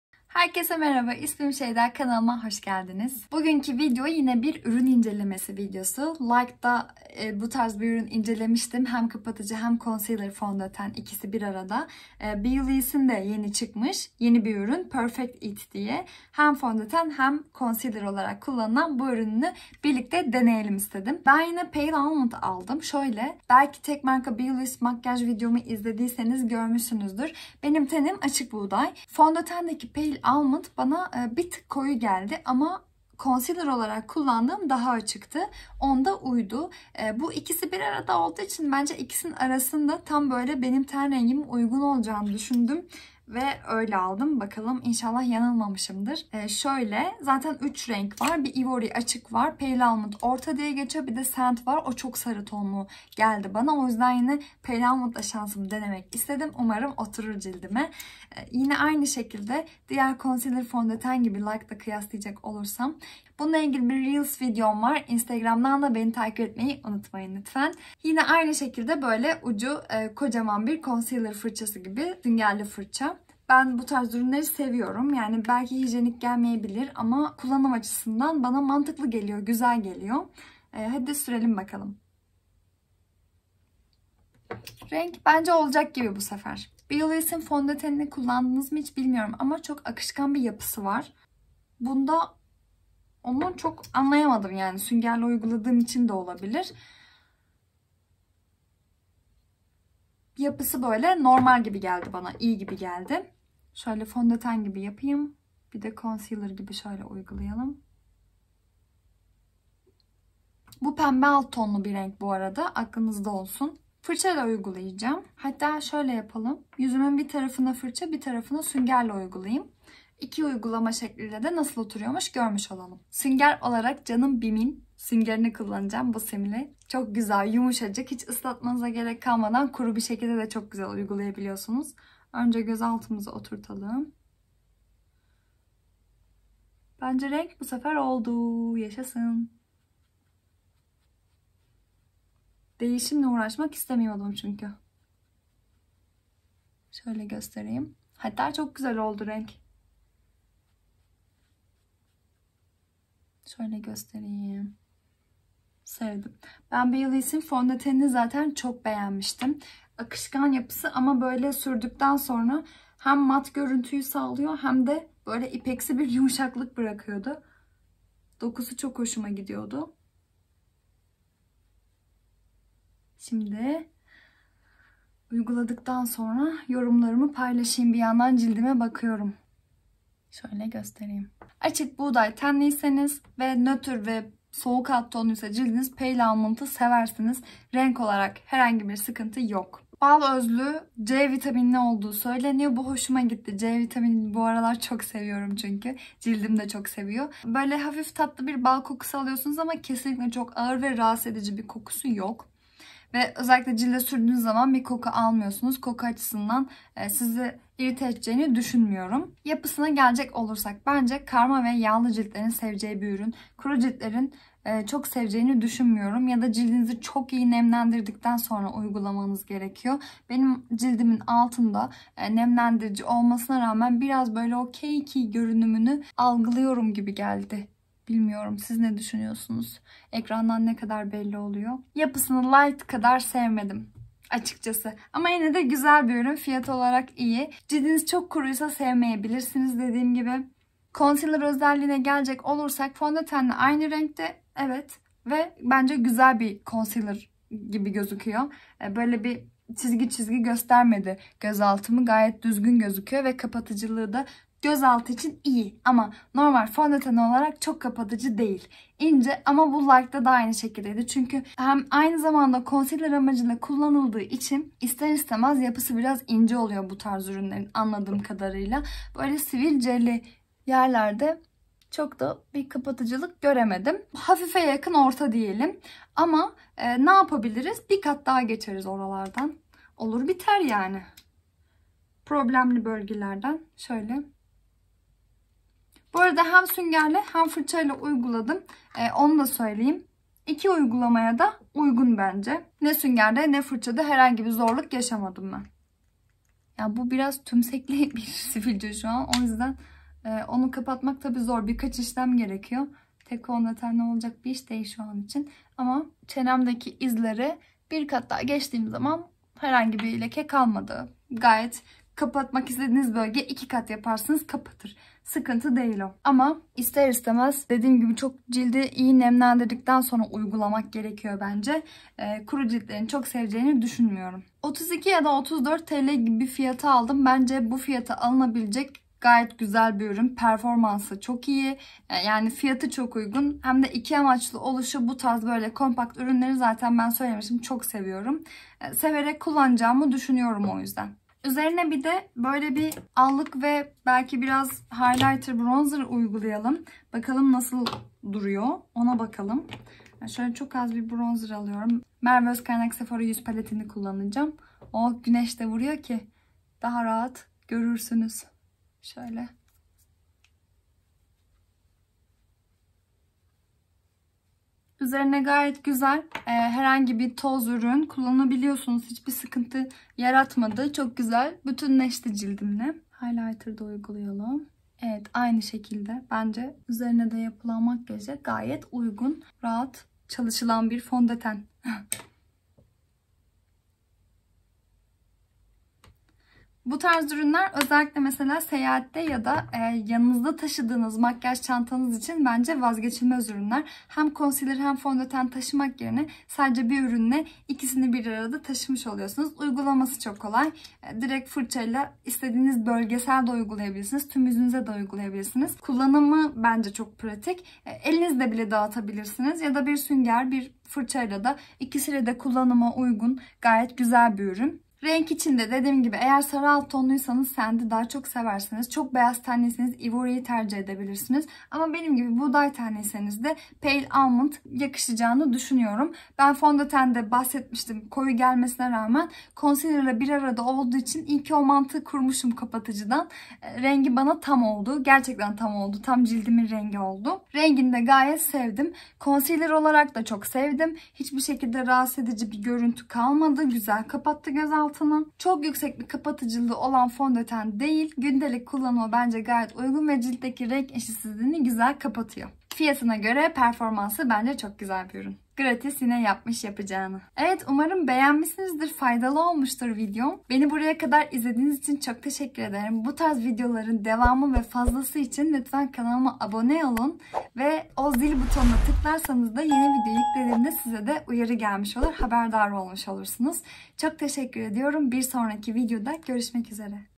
BEAULİS 2 in 1 Fondöten ve Kapatıcı Birlikte Deneyelim | Ürün İncelemesi
Herkese merhaba,
Ürün incelemesi videosu ile yine karşınızdayım. Gratis' in meşhur Beaulis markasının önceden koleksiyon ürünü olarak çıkardığı fondöteni tekrar satışta, yeni ürün olarak geldi. İçeriğinde değişim oldu mu bilmiyorum ben yeni sizlerle denedim bu videoda. Ama ürünün en güzel ve farklı yapan özelliği 2 si 1 arada kapatıcı ve fondöten olarak ikili kullanıma uygun olması. Tek ürünle hem fondöten hem concealer almış oluyorsunuz. Ben yorumlarımı videomda anlattım, siz de yorumlarda paylaşın :)
Ayrıca LYKD markasının 2 si 1 arada fondöten&kapatıcı ürünü ile bu ürünü kıyasladığım bir reels paylaştım instagramda. Instagramda da beni takip ederseniz çok mutlu olurum. Destekleriniz için şimdiden teşekkürler :)